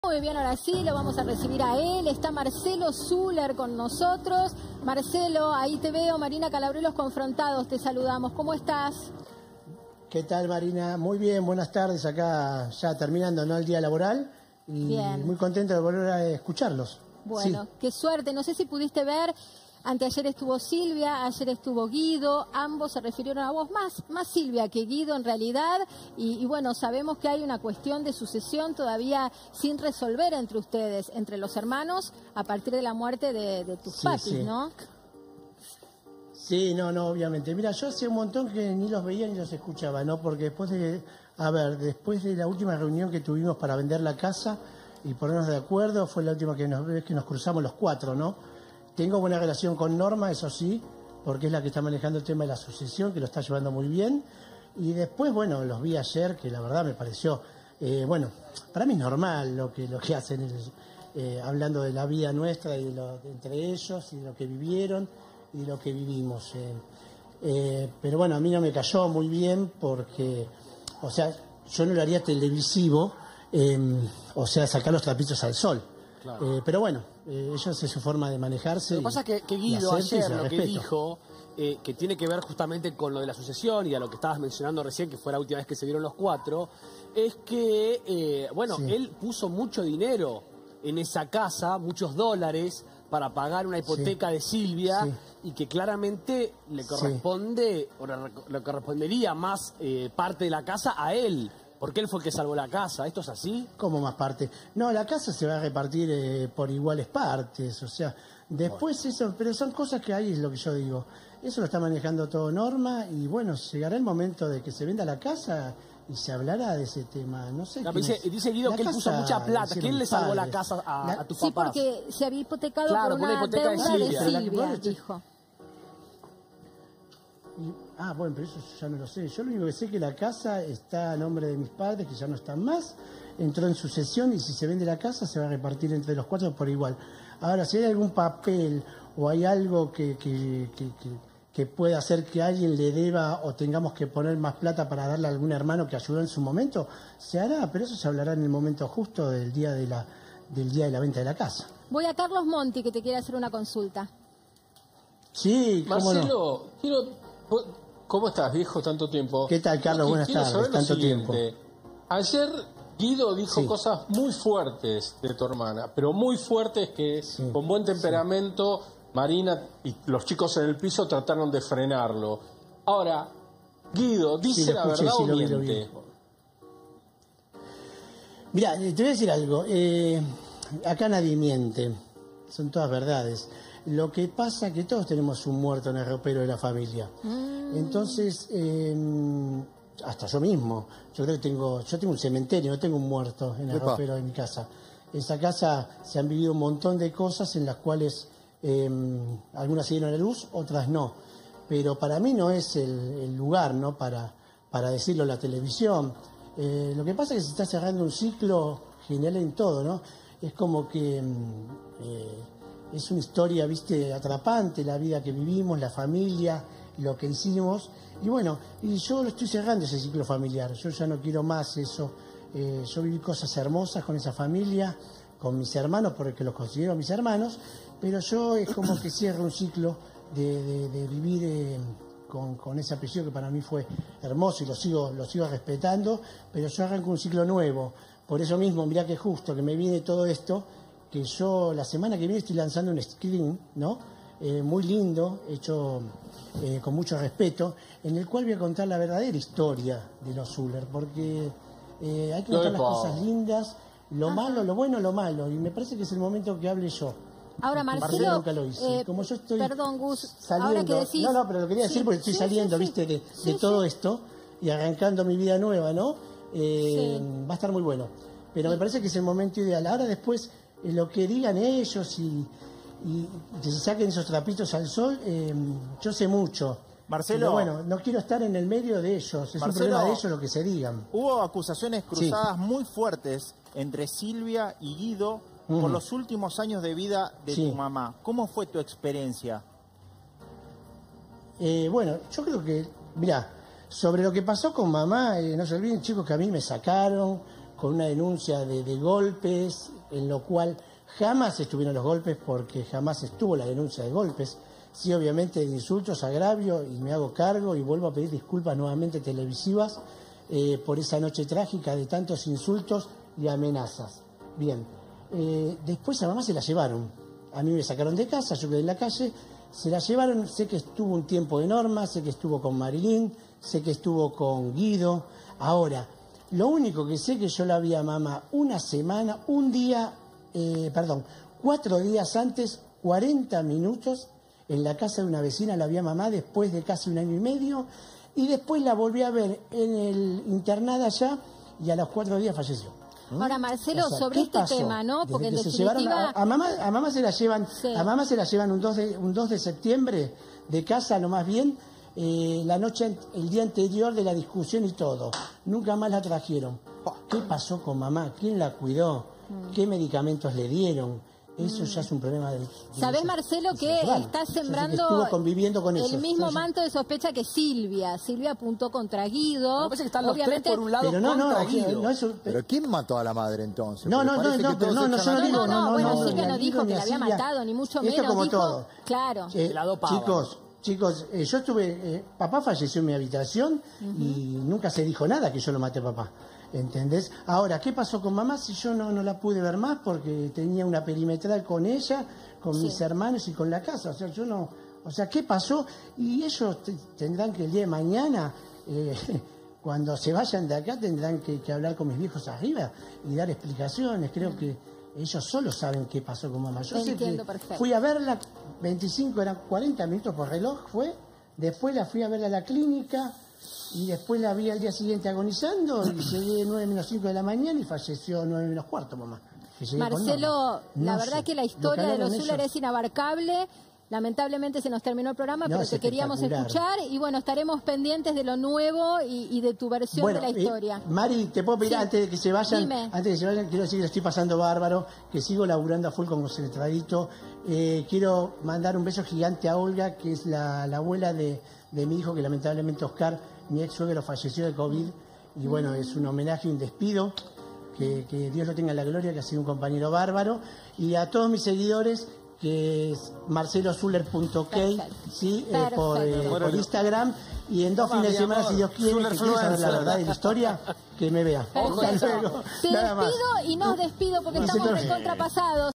Marcelo Süller y el conflicto con sus hermanos
Conversamos con Marcelo Süller para conocer su versión de lo que dijeron Guido y Silvia sobre los conflictos familiares y la sucesión de sus padres.